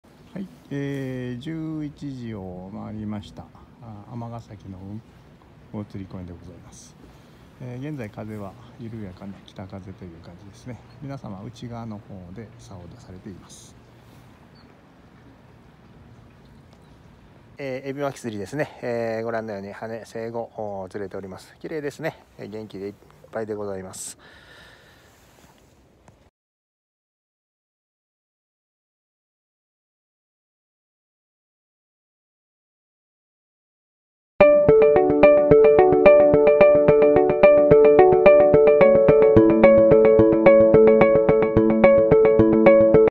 エビ撒きいい感じです♪ fishing max武庫川店
朝の尼崎魚つり公園
エビ撒き釣りいい感じ！
ハネやセイゴ、チヌなど釣れています♪
61cmのスズキも釣れましたよ！